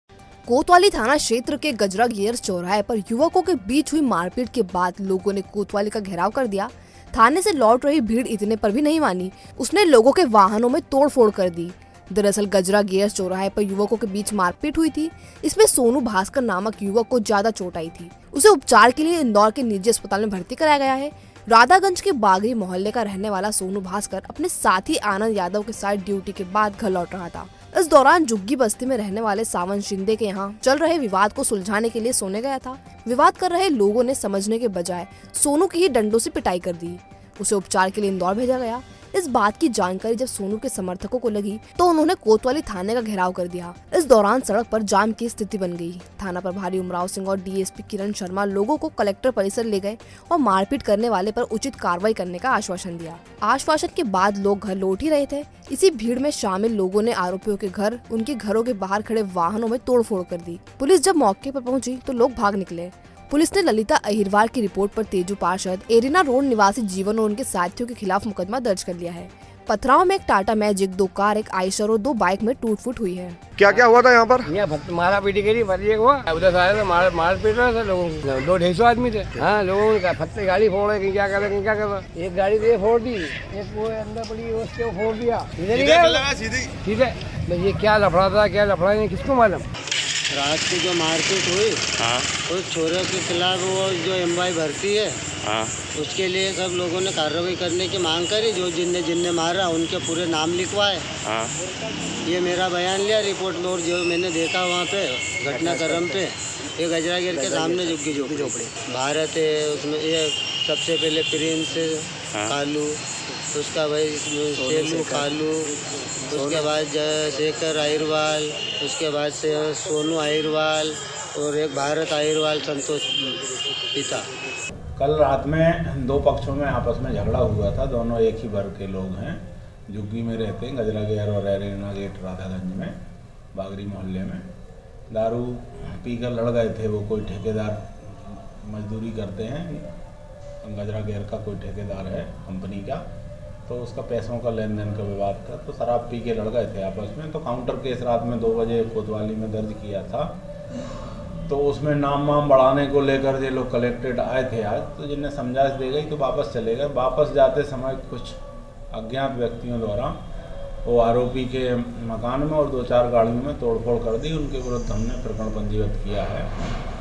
Gajra Gears चौराहे पर दो पक्षों में हुई मारपीट | Dewas | Abhay News
कोतवाली थाना क्षेत्र के गजरा गियर्स चौराहे पर युवकों के बीच हुई मारपीट के बाद लोगों ने कोतवाली का घेराव कर दिया। थाने से लौट रही भीड़ इतने पर भी नहीं मानी, उसने लोगों के वाहनों में तोड़फोड़ कर दी।